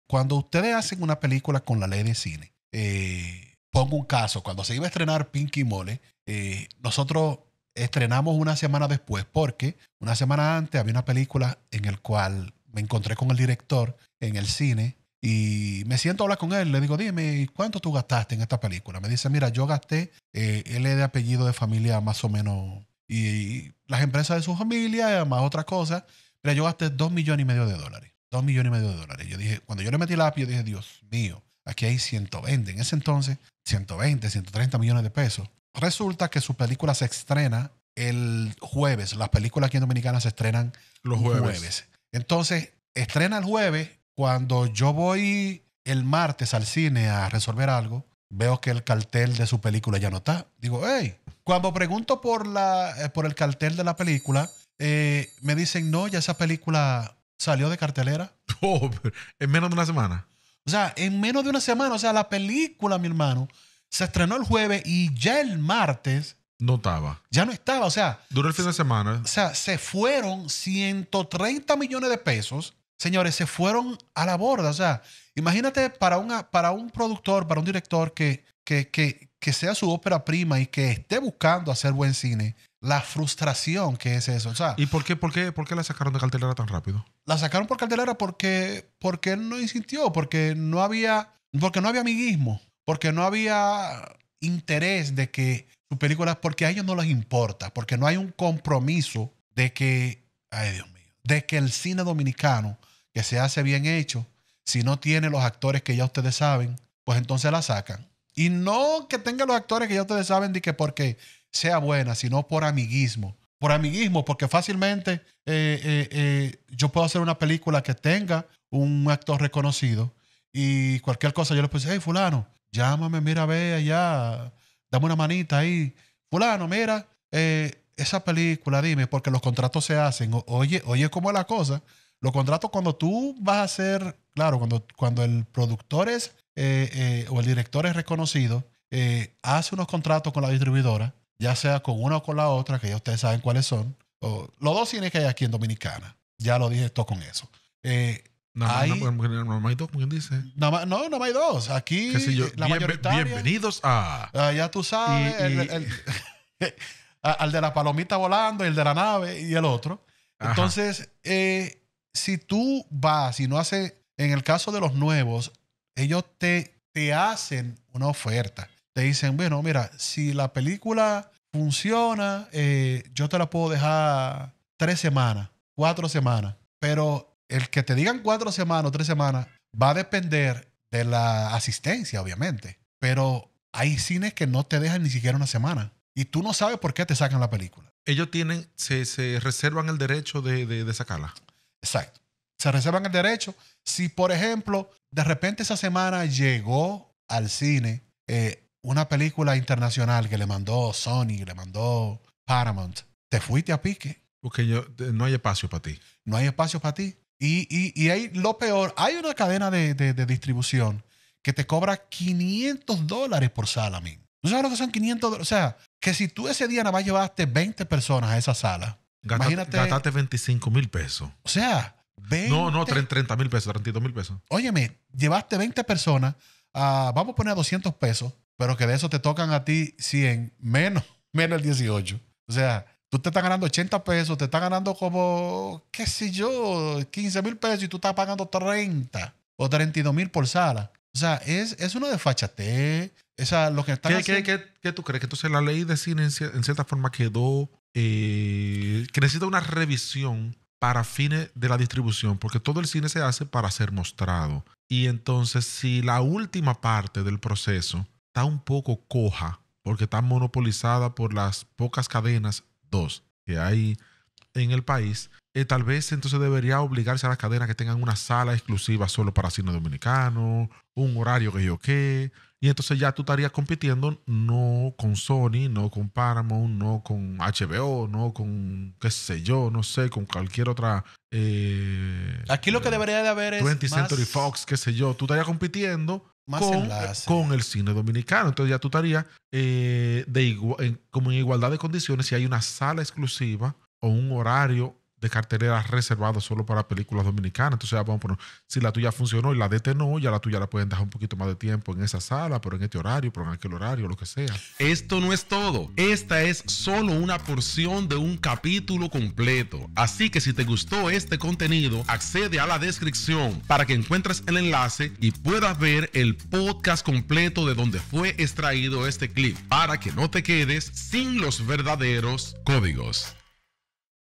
Estrenar una Película es un INFIERNO
Hoy conversamos con Francis Rollings, director de la película Pinky & Mole, y hablaremos de cómo es hacer una película con la ley de cine de la DGCINE, cómo conseguir presupuesto, y entre muchas otros trucos y anécdotas de cine independiente.